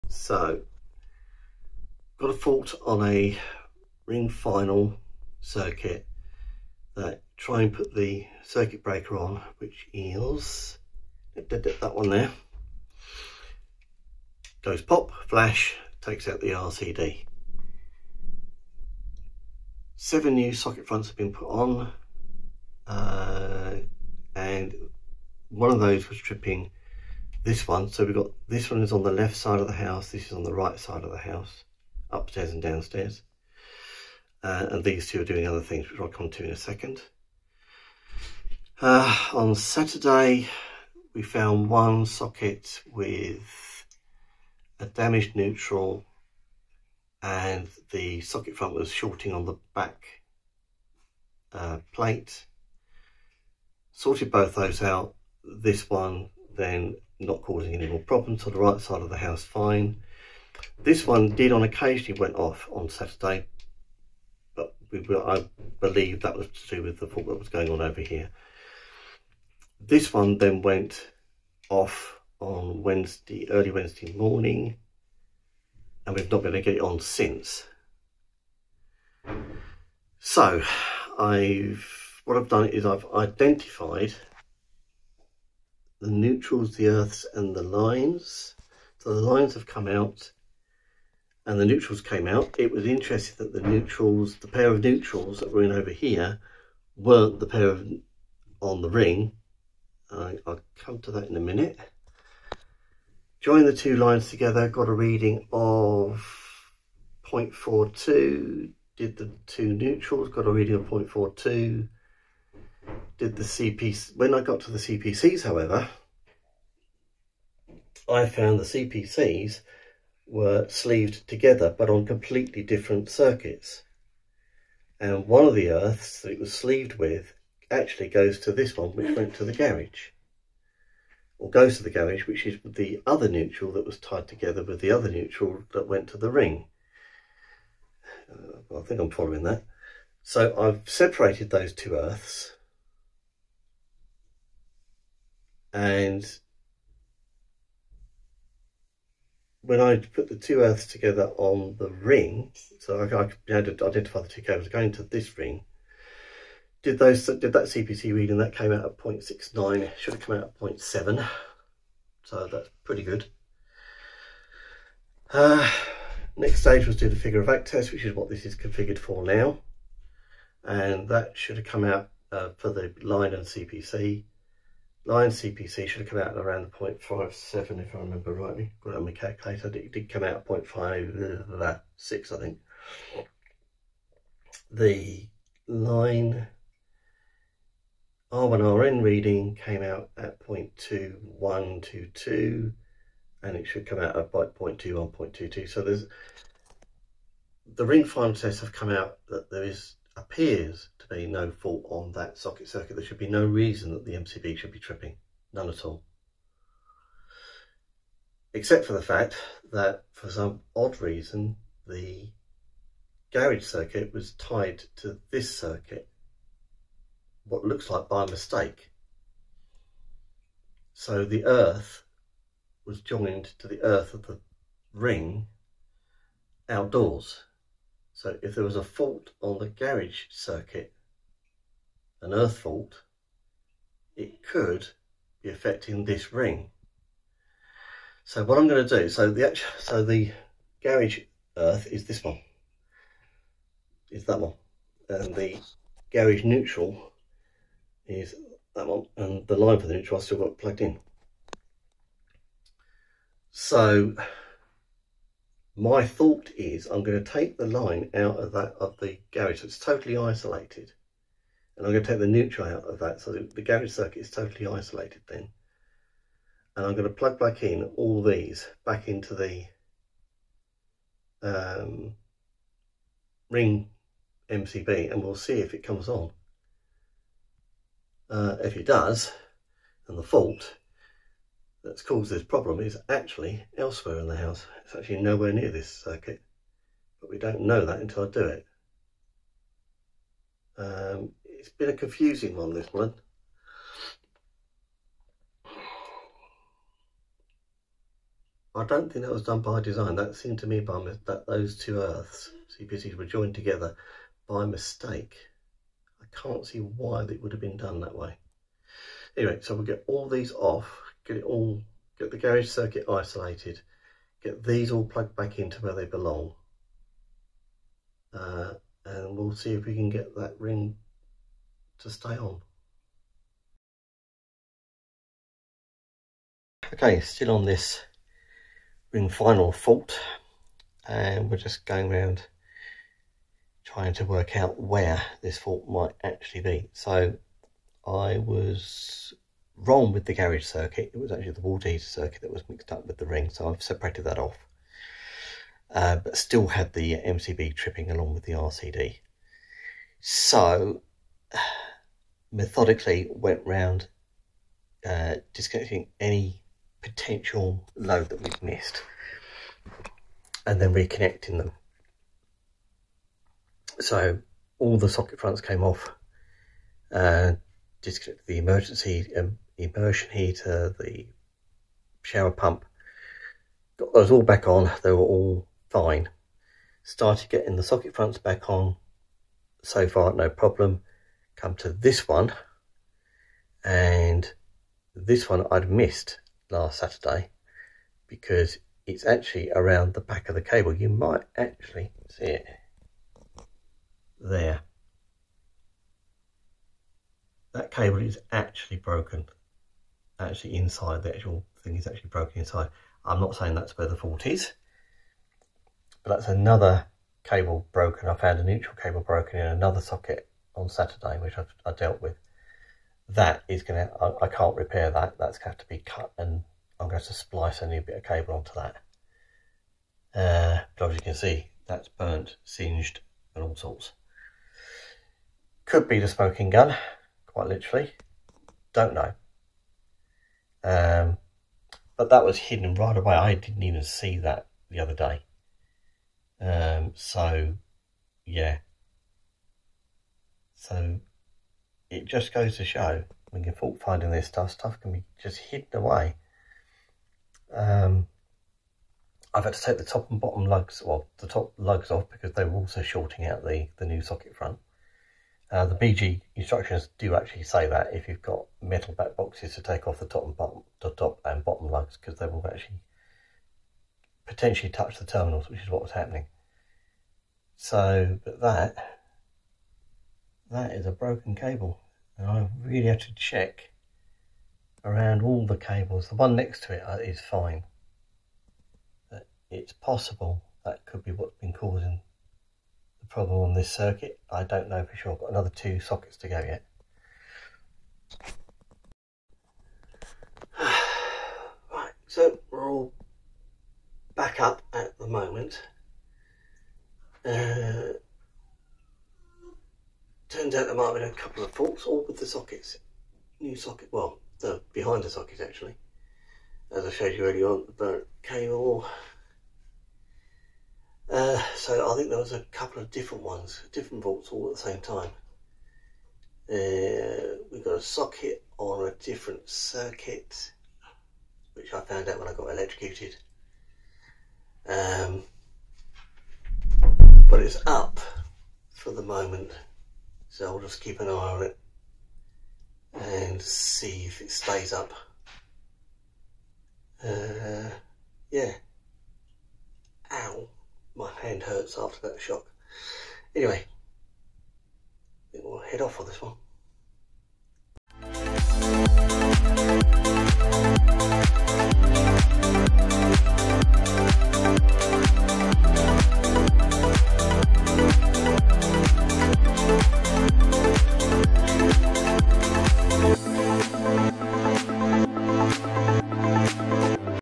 Tripping Time ! Fault Finding a Tricky MCB and RCD Tripping Issue
As with all tripping problems, they are never caused by the same issue, and it's just a case of quizzing the client, and methodically testing everything.

In this case, it turned out to be multiple issues, and a rather painful surprise when one socket turned out to be on a different circuit and was still LIVE !!